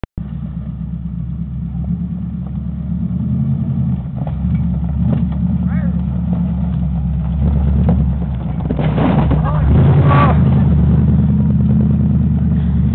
Landcruiser on icy mud hole
Steve taking the Lancruiser on the mud bug while it was frozen over